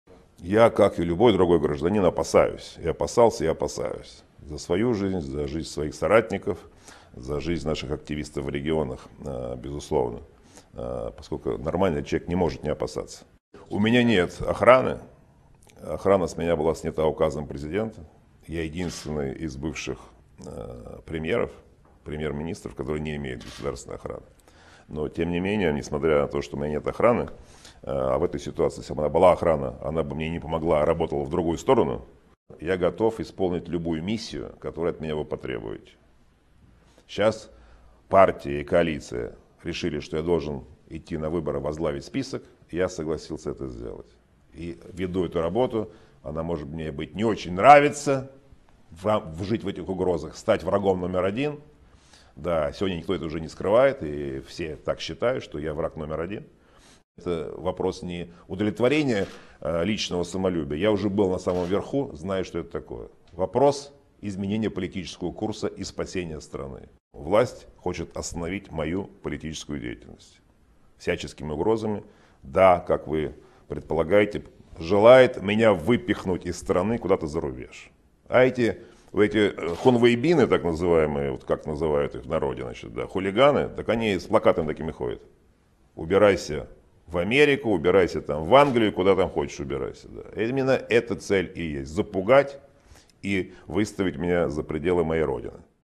Михаил Касьянов. "Быть врагом номер один"
В интервью агентству "Рейтер" Михаил Касьянов рассказал, что регулярно получает угрозы, чувствует себя "врагом номер один", однако, уезжать из России ради собственной безопасности не собирается. С 2000 по 2004 Касьянов занимал пост премьр-министра России. В декабре 2015 стало известно, что политик возглавит оппозиционную Демократическую коалицию на выборах в Государственную Думу 2016 года.